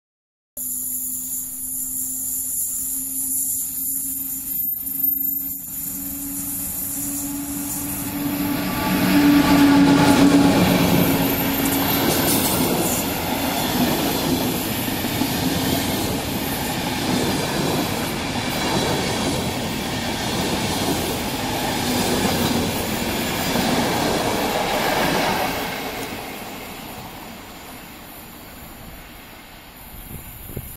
EP09-010 z EIC 1701 Lech na szlaku w kierunku Grodziska Mazowieckiego.
EP09-010 z EIC 1701 Lech relacji: Warszawa Wschodnia - Poznań Główny na szlaku w kierunku Grodziska Mazowieckiego.
Data nagrywania filmu: 5 maja 2013.